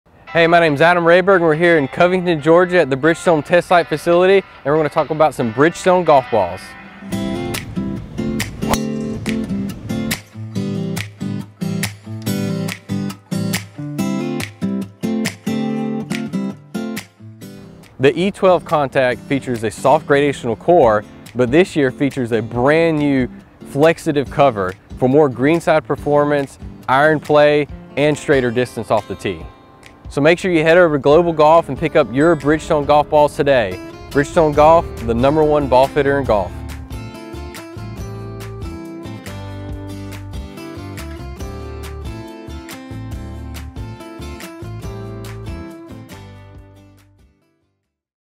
The All-New Bridgestone e12 Golf Ball
Bring out the Bridgestone Golf E12 Contact 2023 Golf Balls 12-Pack and master your favorite course. The FLEXATiV covers and Contact Force Dimples combine to help ensure generous contact with each swing, and the polybutadiene cores and 3-piece construction help you enjoy long, straight distance.